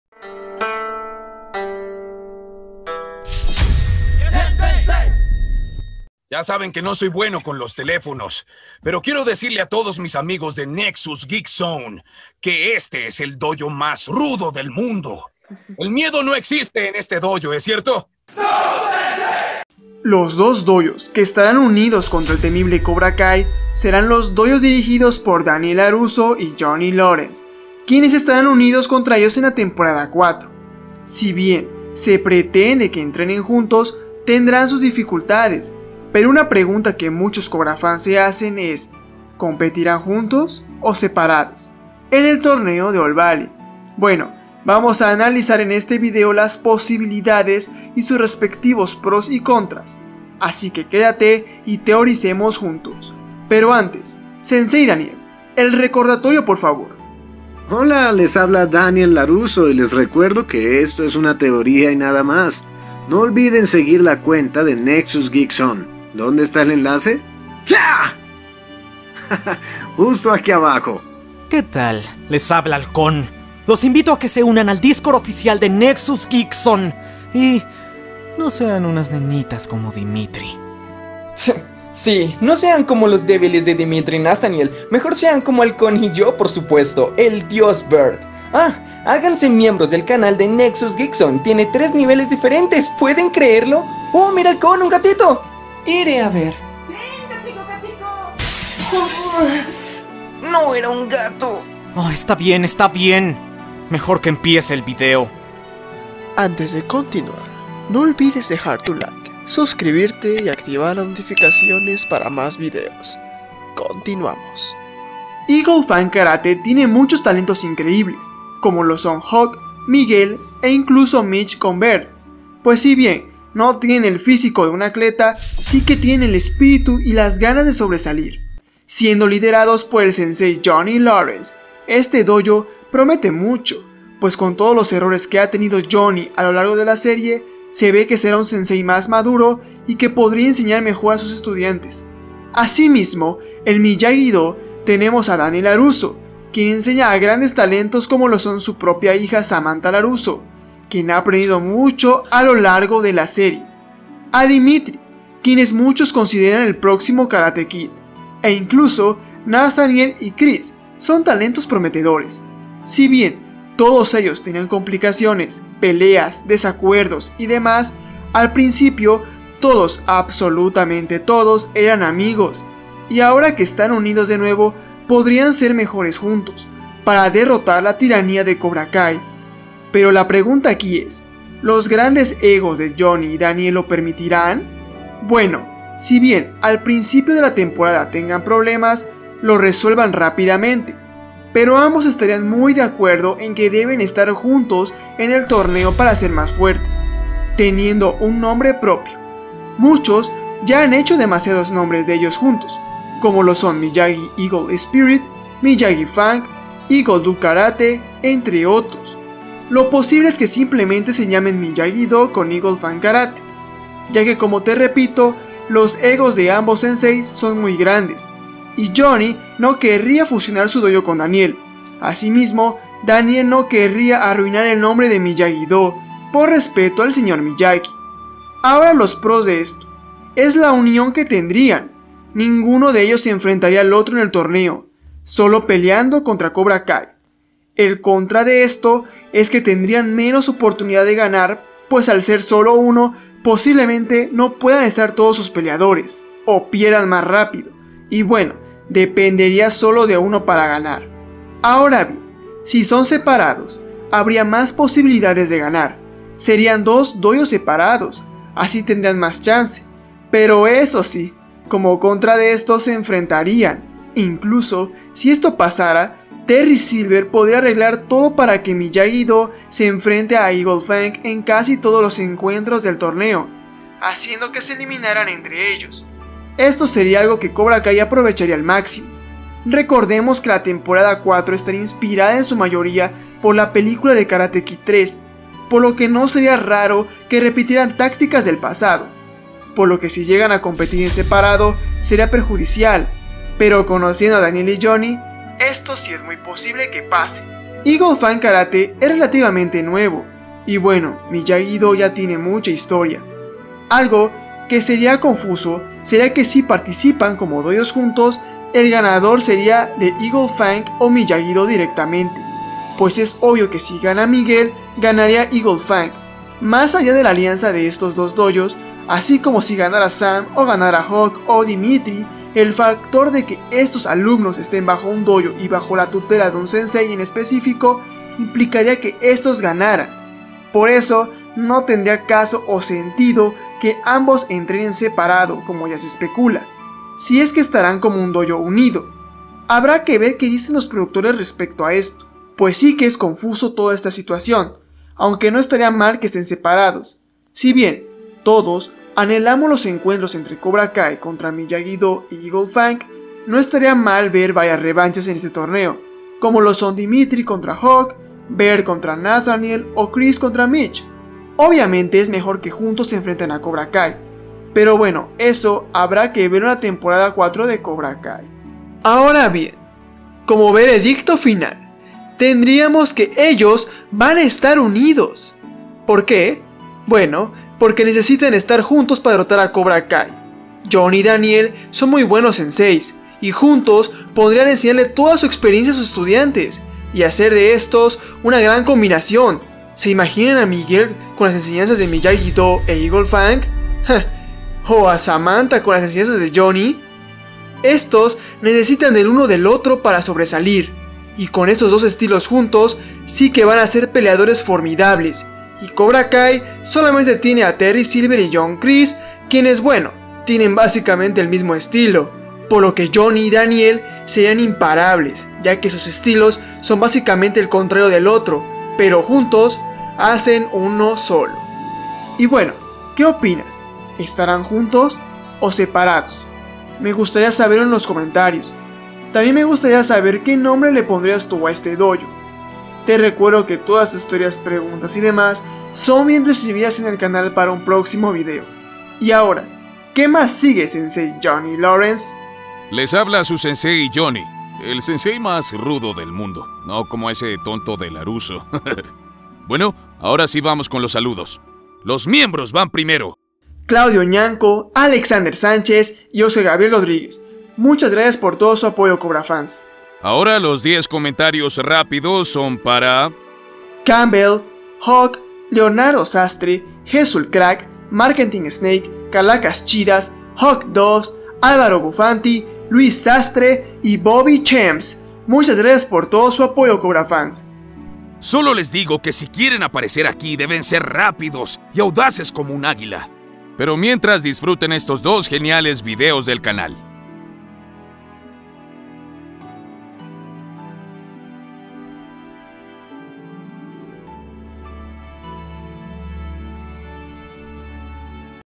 Miyagi Do y Eagle Fang competirán juntos | Pros y Contras de estar unidos | Cobra Kai Temporada 4
Y para ustedes ¿Deberían estar unidos o separados? déjamelo saber en los comentarios